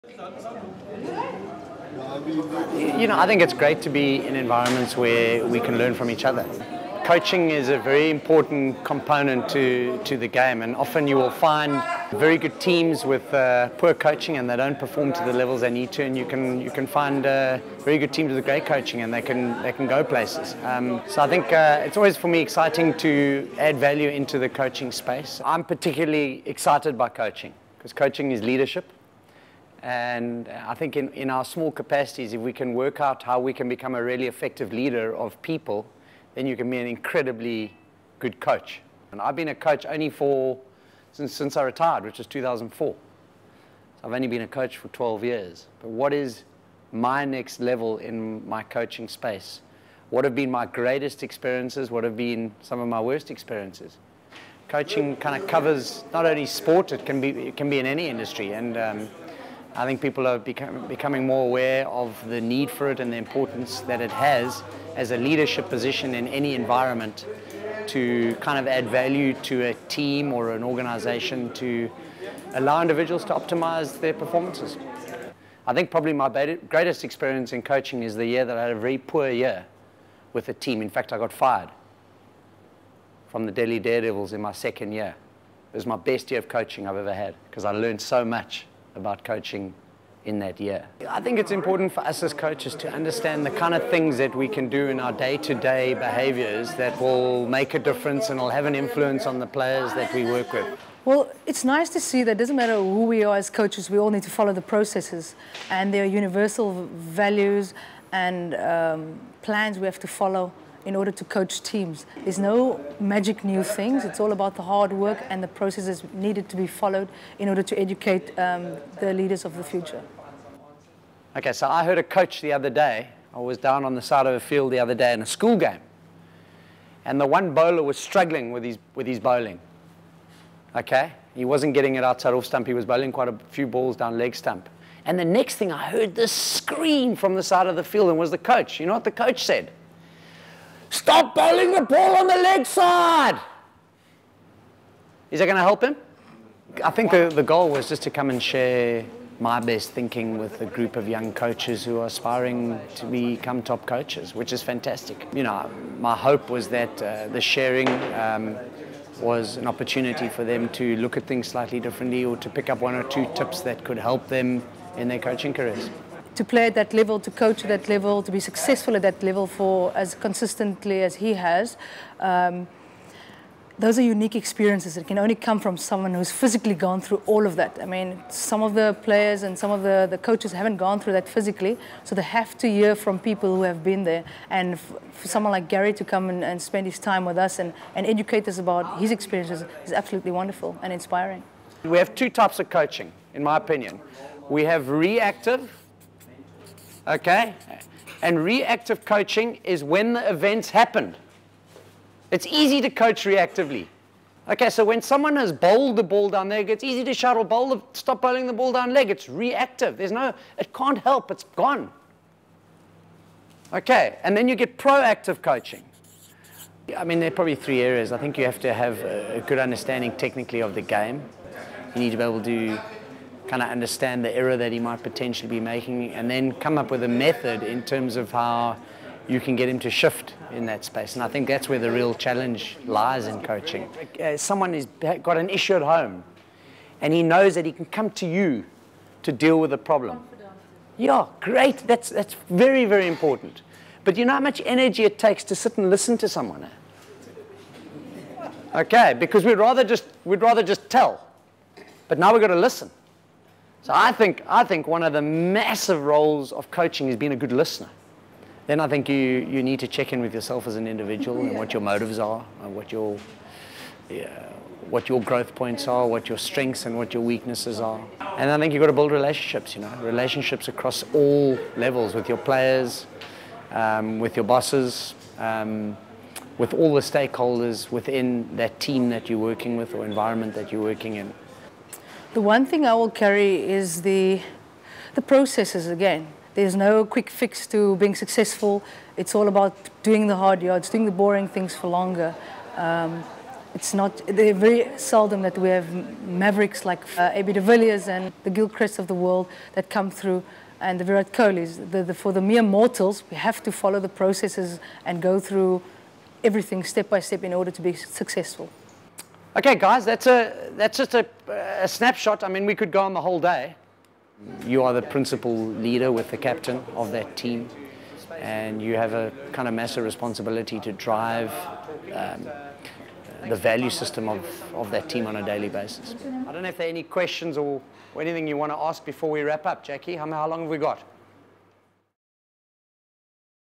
GARY KIRSTEN AUDI 'COACHES CORNER' WORKSHOP
Gary Kirsten hosted the Audi 'Coaches Corner' workshop in Cape Town and Johannesburg. This workshop aimed at providing developmental coaches with the necessary insight and skills to become better coaches and transcend the sport to greater heights within those areas.